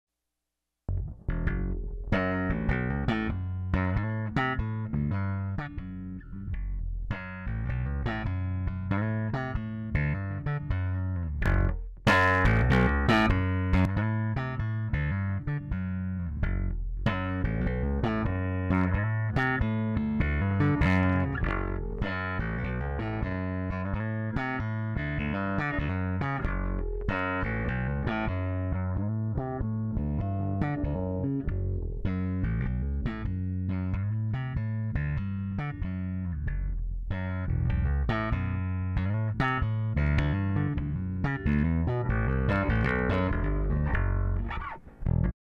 Daring Audio Phat Beam Compressor / Limiter knob turning demo (VCA = on, Low Pass Shape Filter) z22
We start with a moment of dry signal recorded too hot, and note that you get some digital distortion.  We reamp this signal through the Daring Audio Phat Beam, and use the limiter, compressor and filter all simulteneously. Here the VCA = on, (right toggle switch on), and the left toggle switch is Green, down, and therefore the Shape filter is low pass filter.  We move the Shape filter so you can here the changes in cut off frequency. Note the Blend knob is to the far right, indicating nearly all wet signal.  You can of course, move the Blend toward the center so that you mix some dry signal in. This lessons the effect, and makes it sound more natural if you want.  It also bring back the attack of the string, which is softened by the compresor.